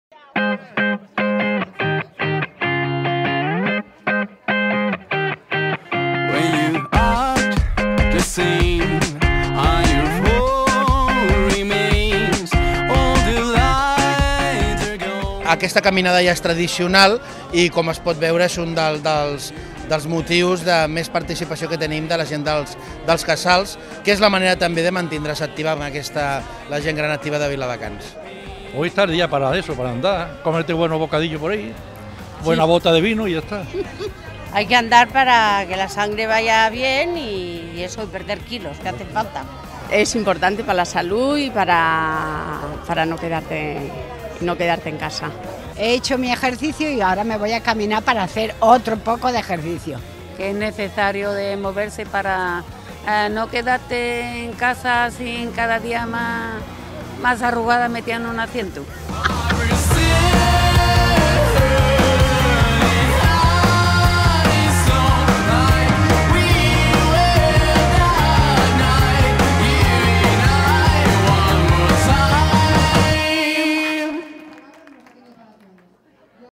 Gent Gran amb molta marxa
La 17a Marxa de la Gent Gran del Baix Llobregat commemora el seu Dia Internacional. El recorregut suma uns cinc quilòmetres amb inici a la Plaça de la Vila i final a la Plaça de la Constitució.